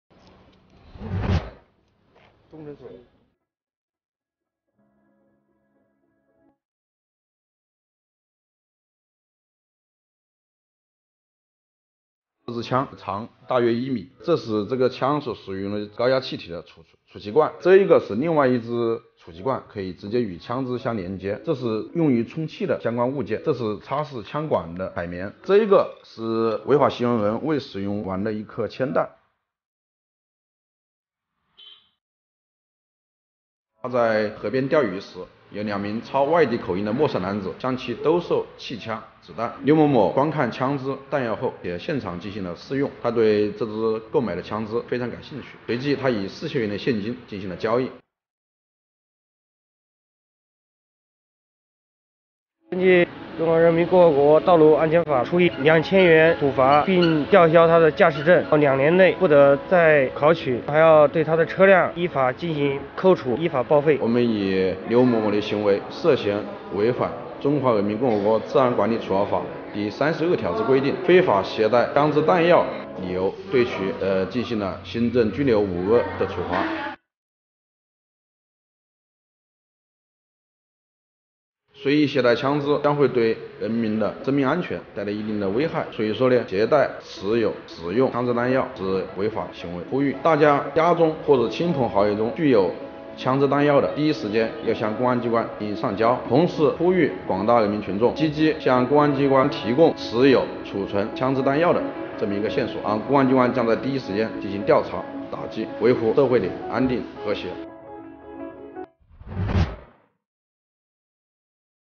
报废车上路被查搜出1米长气枪，车主：钓鱼时花4000元购得
近日，湖北随州。男子刘某开报废车辆上路行驶被查，交警竟从其车内搜查出一把高压气枪。随后，交警将刘某移送到派出所，经鉴定，这支气枪有很强的杀伤力，操作不当会造成严重后果。Recently, Suizhou, Hubei Province. The man Liu drove a scrapped vehicle on the road and was checked. The traffic police searched a high-pressure air gun from his car. Subsequently, the traffic police transferred Liu to the police station. After identification, this air gun has strong lethality, and improper operation will cause serious consequences.